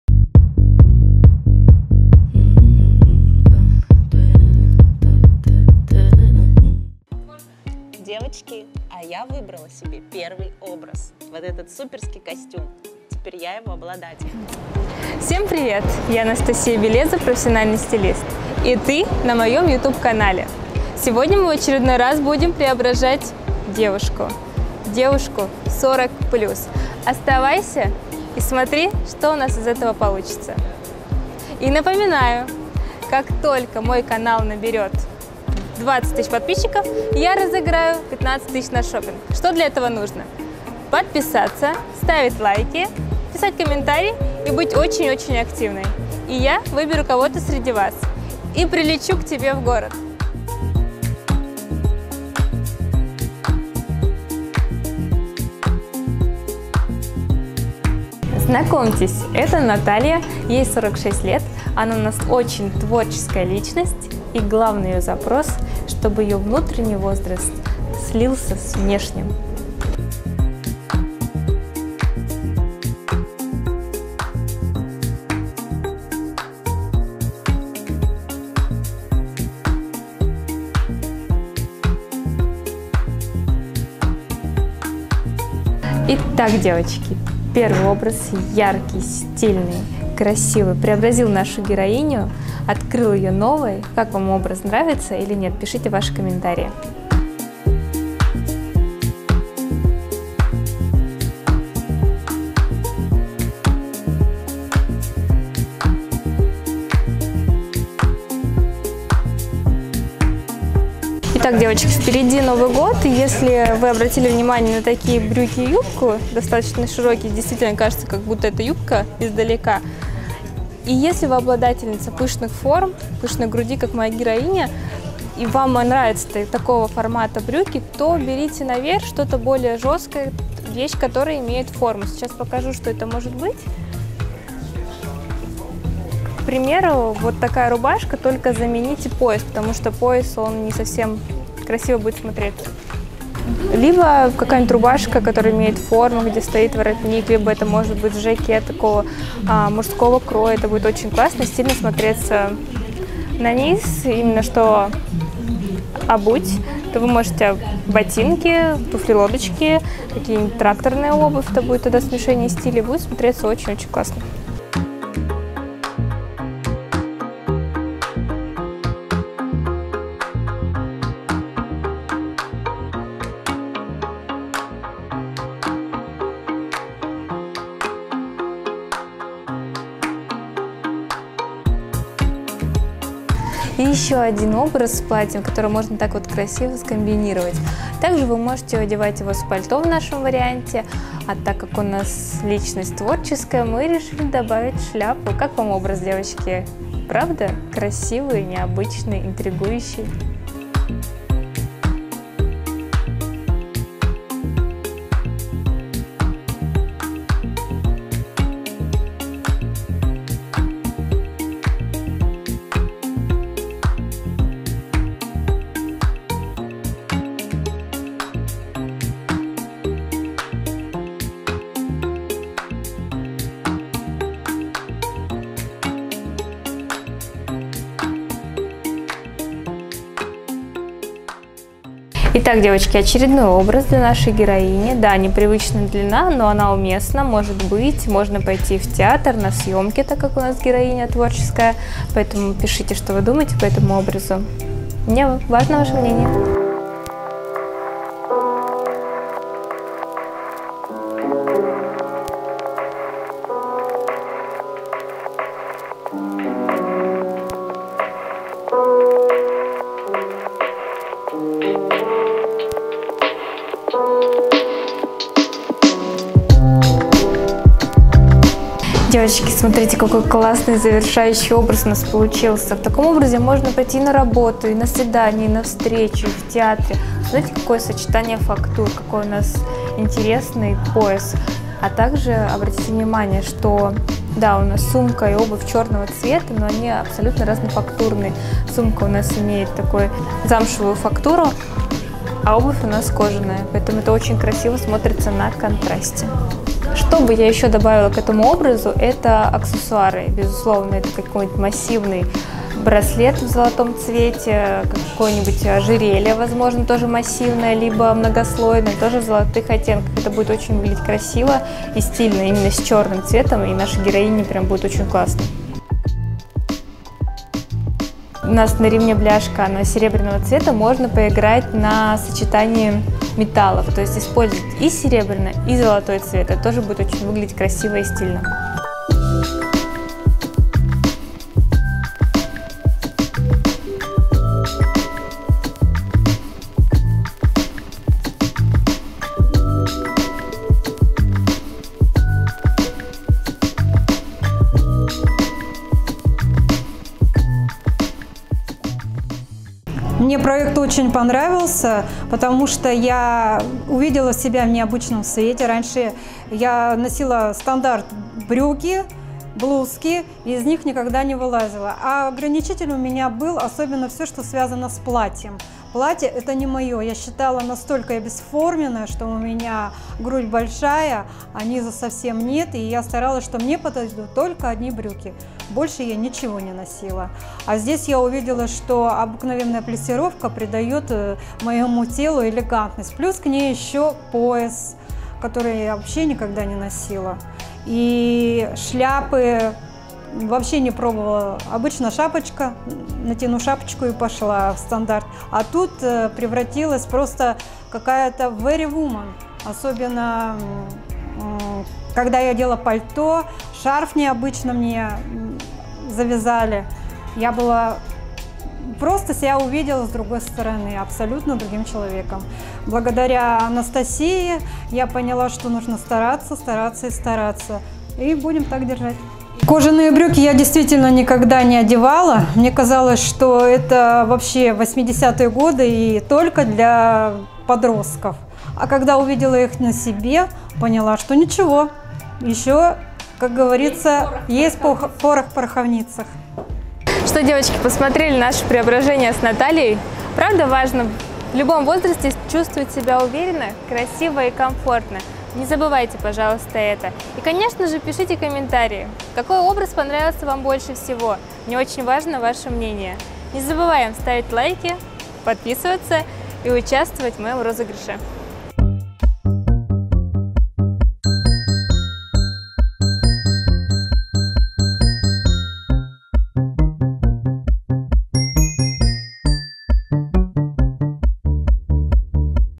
Меняйся вместе с гардеробом. Одежда: платье, юбка и свитер vs кожаные штаны и жакет в h&m.
Смотри мой ютуб канал и ты узнаешь: как проводит свои будни стилист, как совершать покупки одежды, как выглядеть красиво.

Данный ролик посвящен перевоплощению новой героини - Натальи. Одежда, обувь и аксессуары представлены в магазине h&m. Героиня примеряет на себе смелые образы: кожаные штаны и жакет, учится смотреть на себя по другому в платье, даже юбка и свитер сидят по иному. Пиши какой образ понравился именно тебе и возможно героем главного выпуска с подарками)) будешь именно ты.